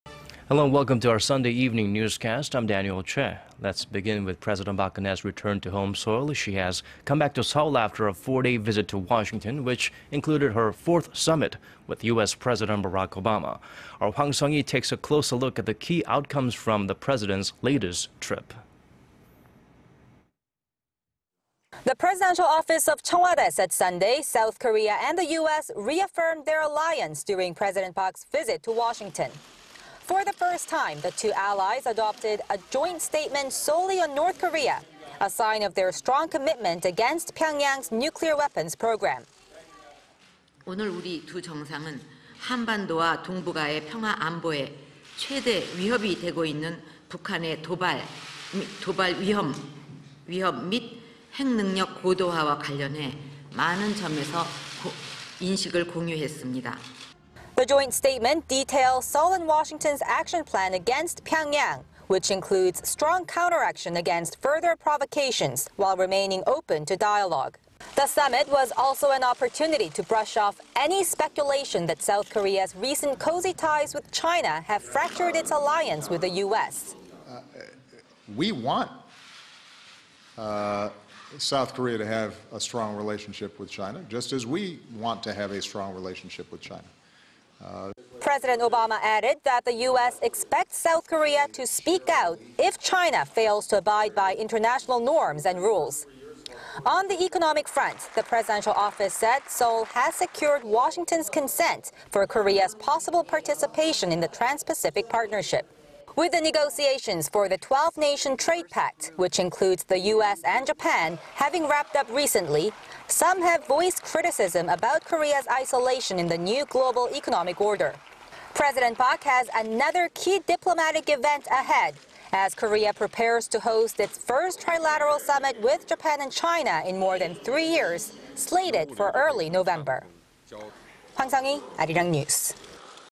S. Korea， U.S. reaffirm alliance during Pres. Park′s Washington visit 박대통령 방미
We begin with President Park Geun－hye...
She has returned to Seoul after a four－day visit to Washington， which included her fourth summit with U.S. President Barack Obama.
Our Hwang Sung－hee takes a closer look at the key outcomes from the president′s latest trip.
The presidential office of Cheong Wa Dae said Sunday South Korea and the U.S. reaffirmed their alliance during President Park′s visit to Washington.
For the first time， the two allies adopted a joint statement solely on North Korea， a sign of their strong commitment against Pyongyang′s nuclear weapons program.
″We， the two leaders， shared many ideas about North Korea′s provocations and nuclear development program， which pose the biggest security threat to the peace on the Korean Peninsula and in Northeast Asia.″
The joint statement details Seoul and Washington′s action plan against Pyongyang， which includes stronger counteraction against further provocations while remaining open to dialogue.
The summit was also an opportunity to brush off any speculation that South Korea′s recent cozy ties with China have fractured its alliance with the U.S.
″We want South Korea to have a strong relationship with China， just as we want to have a strong relationship with China.″
President Obama added that the U.S. expects South Korea to speak out if China fails to abide by international norms and rules.
On the economic front， the presidential office said Seoul has secured Washington′s consent for Korea′s possible participation in the Trans－Pacific Partnership.
With the negotiations for the 12－nation trade pact  which includes the U.S. and Japan  having wrapped up recently， some have voiced criticism about Korea′s isolation in the new global economic order.
President Park has another key diplomatic event ahead，... as Korea prepares to host its first trilateral summit with Japan and China... slated for early November.